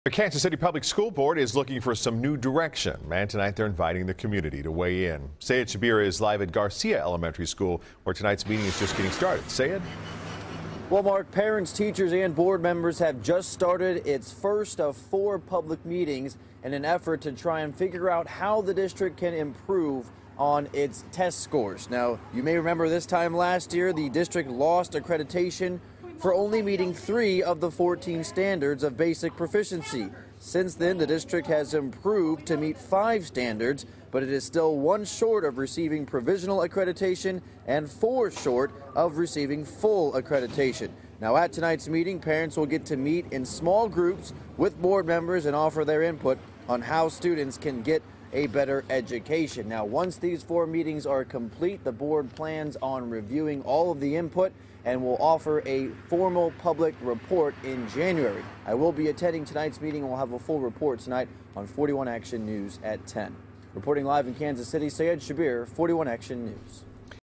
KC Public Schools seek community input
Description: The Kansas City Public Schools will host its first of four public meetings Tuesday night to gain community input on how to improve the district.